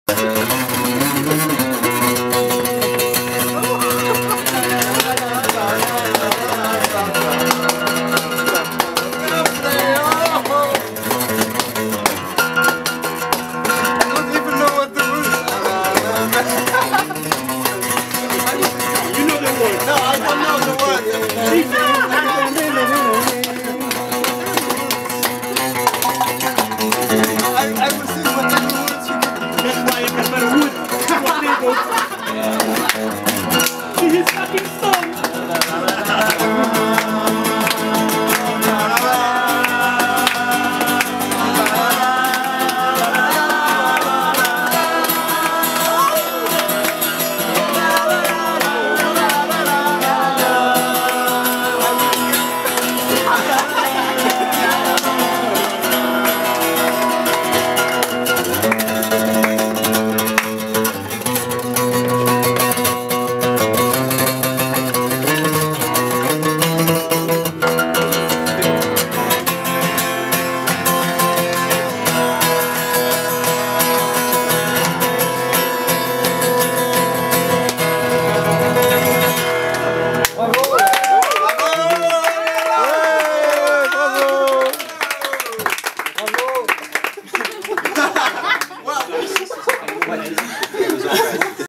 Musical Highlights from Valuetools 2012 Conference (ii)
Julien Gaillard presenting a Guitar concert